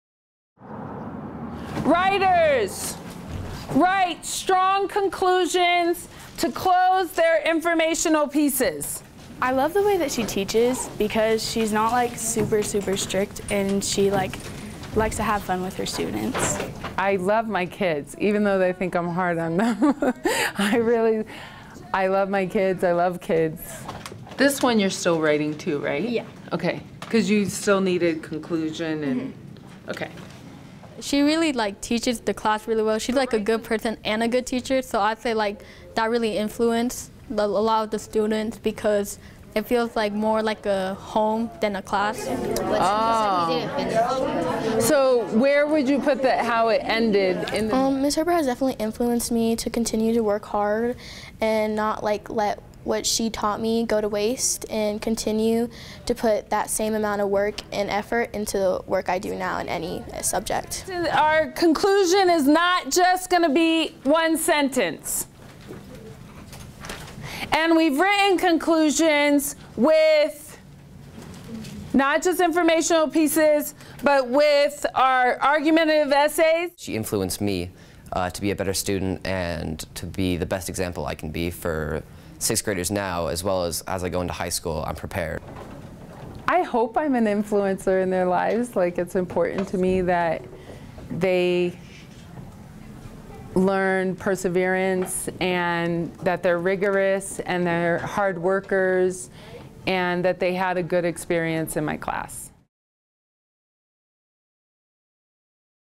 Teacher Appreciation Week - Meet Rebecca Herbert
Many students follow "influencers" on social media. However, it turns out the REAL influencers in students' lives aren't the ones on TikTok or Instagram - they're the ones right in front of them every day. Meet real-life influencer Rebecca Herbert, a 6th grade Language Arts teacher at Fox Ridge Middle School.